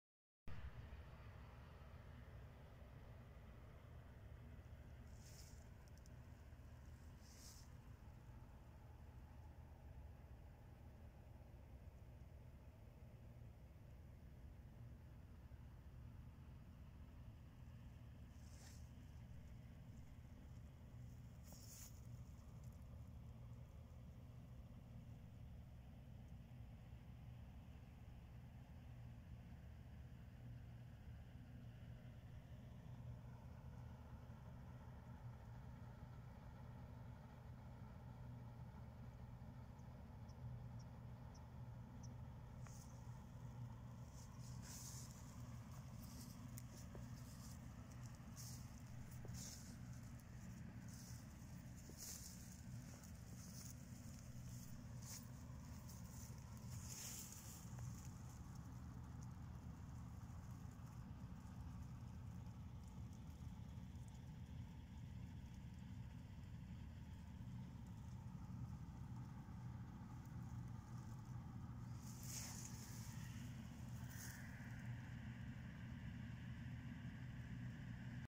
Part 2: Finnish Sunflower Field
Most of the sunflowers, but not all, are blooming. September 1, 2020.